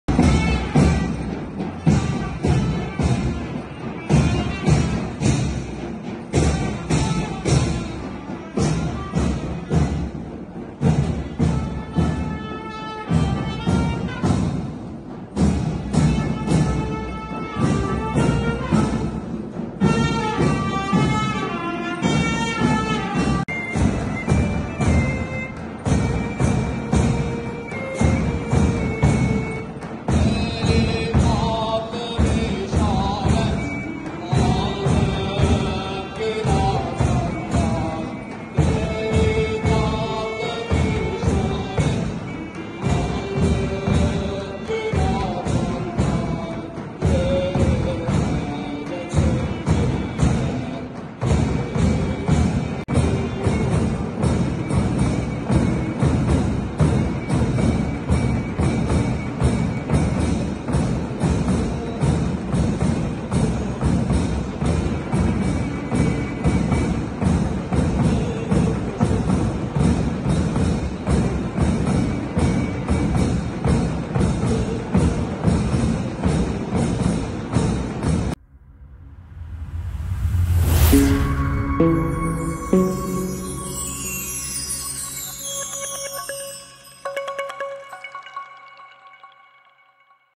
Cadde54 AVM de Pamukova Mehteran Rüzgarı..
Pamukova Anadolu İmam Hatip Lisesi Mehter Takımı  Cadde54 AVM de  düzenlediği mehteran gösterisiyle  izleyenleri büyüledi..